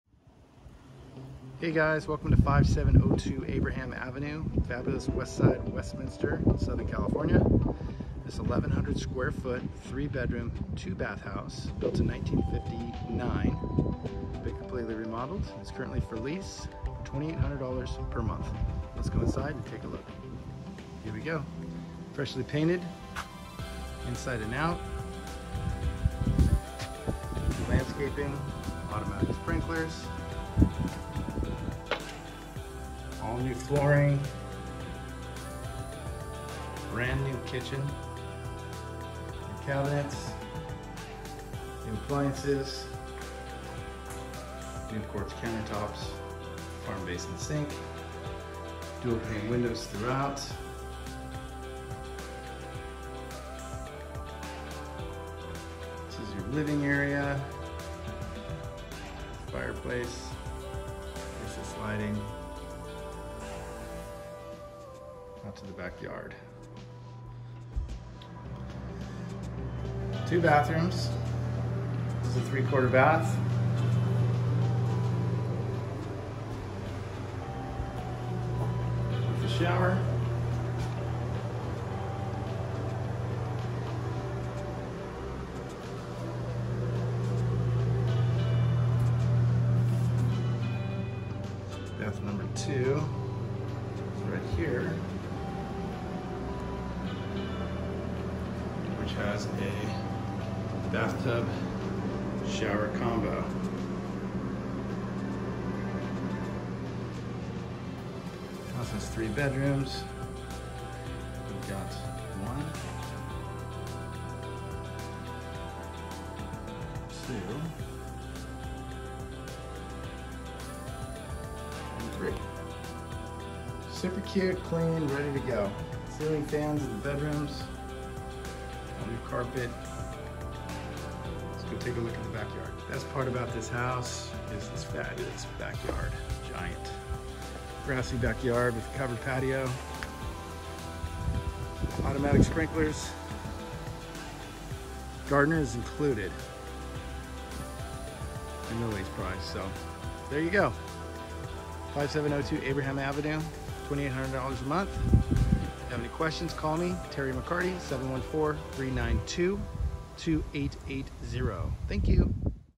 5702 Abraham Ave. Westminster Ca
Completely remodeled 3 Bedroom 2 Bath Single Family Home with brand new kitchen and a giant grassy backyard. Property is located on the Westside of Westminster bordering Seal Beach and Huntington Beach. Walking distance to Eastwood Elementary School and Bolsa Chica Park!